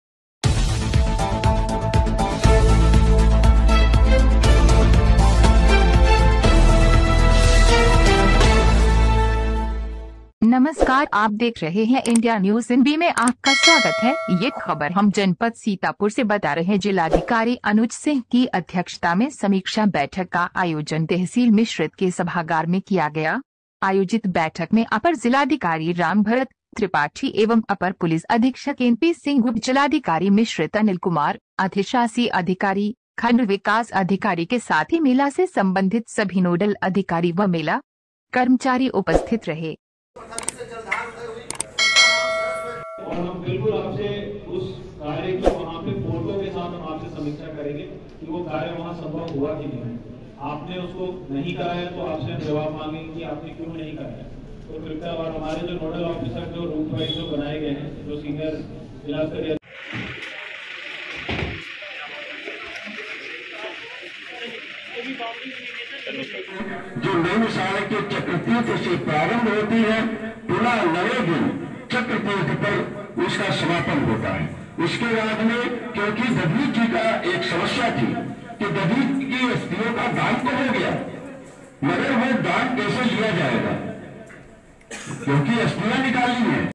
जनपद सीतापुर जनपद कस्बा मिश्रित में होने वाली विश्व विख्यात 84 कोसीय धार्मिक होली परिक्रमा को सकुशल
सीतापुर  जनपद कस्बा मिश्रित में होने वाली विश्व विख्यात 84 कोसीय धार्मिक होली परिक्रमा को सकुशल संपन्न कराने हेतु
 ब्यूरो रिपोर्ट सचिन शुक्ला

जिलाधिकारी अनुज सिंह की अध्यक्षता में  समीक्षा बैठक का आयोजन तहसील मिश्रित के सभागार में किया गया । आयोजित बैठक में अपर जिलाधिकारी राम भरत त्रिपाठी एवं अपर पुलिस अधीक्षक एनपी सिंह  उपजिलाधिकारी मिश्रित अनिल कुमार , अधिशासी अधिकारी , खंडविकास अधिकारी के साथ ही मेला से संबंधित सभी नोडल अधिकारी व मेला कर्मचारी उपस्थित रहे । दिनांक 20 फरवरी को अमावस्या है । भोर होते ही पहला आश्रम से डंका बजते ही चक्रतीर्थ में स्नानों परान्त 84 कोसीय होली  परिक्रमा मेले का शुभारम्भ हो जाएगा । सभी परिक्रमार्थी प्रथम पड़ाव कोरौना के लिए प्रस्थान करेगे । मेले की ब्यवस्थाओं को लेकर जिलाधिकारी ने सभी नोडल अधिकारियों के साथ समीक्षा बैठक की । और पड़ाव स्थलों पर व्यवस्थाओं को लेकर निर्देश दिए । सभी अधिकारियों ने मौसम खराब होने का हवाला देकर एक सप्ताह में सभी कार्य पूर्ण कराने की बात कही है । आयोजित बैठक में 84 कोसीय परिक्रमा पड़ाव प्रभारी विमल मिश्रा ने अव्यवस्थाओं के संबंध में जिलाधिकारी को जानकारी दी । उन्होंने बताया रामगढ़ से लेकर ककरघटा मार्ग तक मार्ग खराब है जलभराव है । पहला आश्रम तक मार्ग खराब हैं । शौचालय की कोई व्यवस्था नहीं है । ककरघटा घाट पर गोमती नदी में परिक्रमार्थी  स्नान करते हैं । कोई अस्थाई घाट नहीं है । वहां अस्थाई घाट बनवाया जाय । तथा गोताखोरों को नियुक्त किया जाय ।

 महर्षि दधीचि की पावन तपो भूमि पर कस्बा मिश्रिख में 5 दिनों तक पंचकोसी परिक्रमा होता है । सभी संत महंत 5 दिनों तक अपना टीकासन लगाकर परिक्रमा करते हैं । परंतु तहसील के पीछे ही स्थित रामबाग में पूरे परिसर में जल भराव की स्थिति बनी हुई है । सिधौली रोड से परसौली मार्ग तक लोग परिक्रमार्थी रुकते हैं वहां कोई प्रकाश की व्यवस्था नहीं है ।  आयोजित बैठक में सभी बिंदुओं पर ध्यान देते हुए जिलाधिकारी ने स्पष्ट रूप में कहा कि एक सप्ताह बाद किसी की बात नही सुनी जाएगी । सभी ब्यवस्थाऐं दुरुस्त होनी चाहिए न होने पर कार्यवाही की जाएगी ।